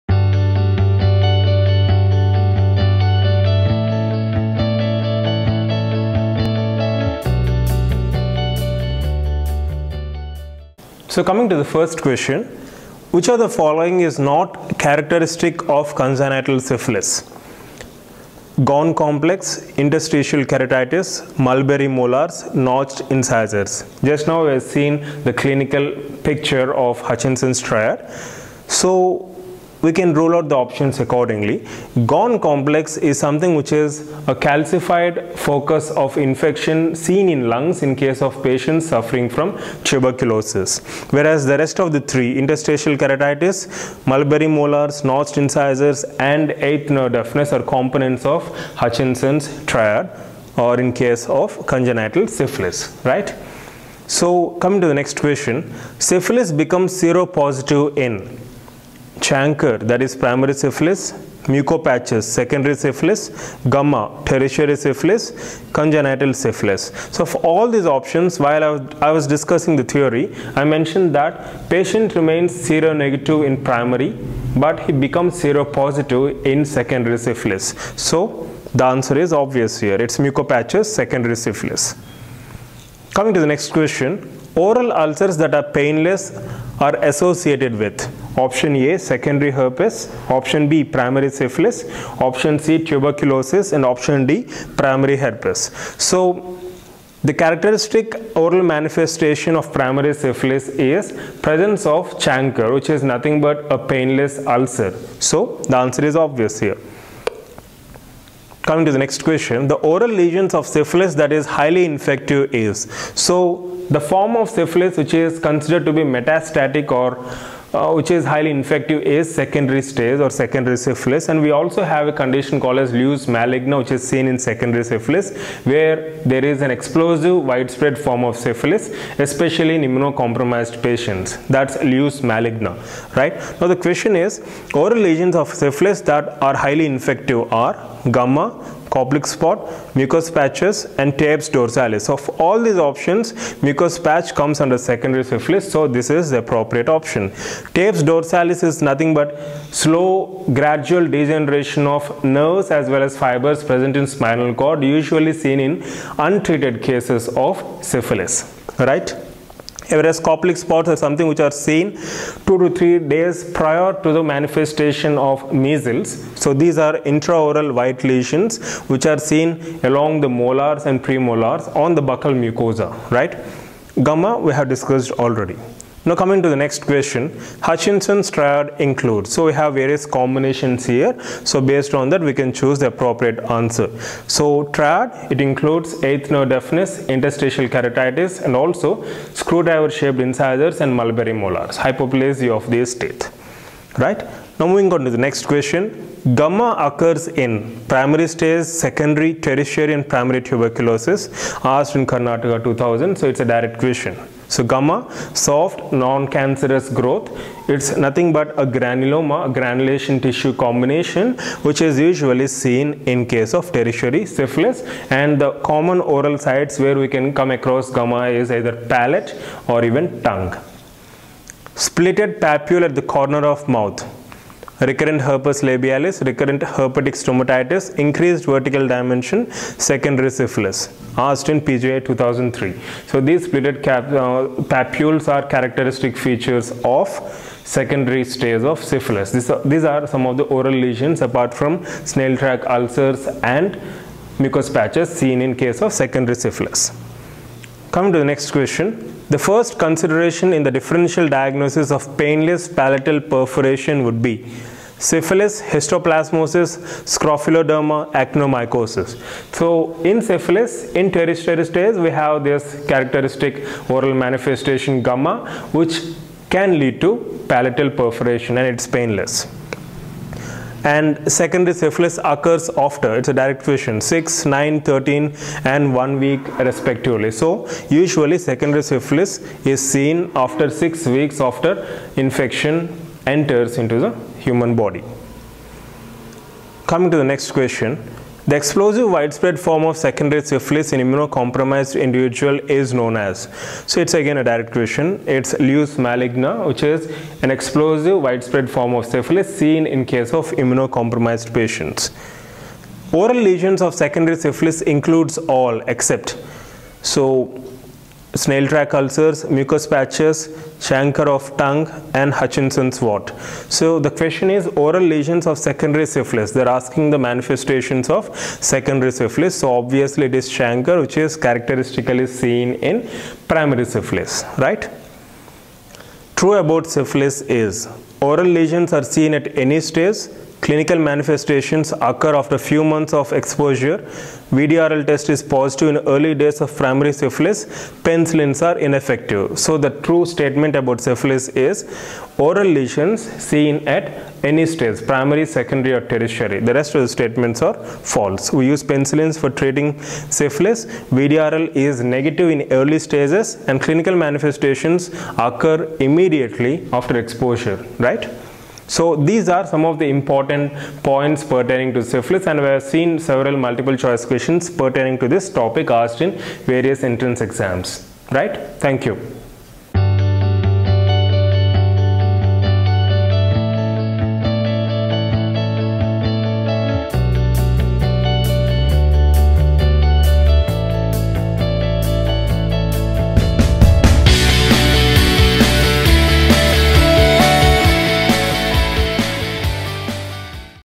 Syphilis - Oral Manifestations
In this video I will be discussing various MCQs in syphilis with focus on oral manifestations.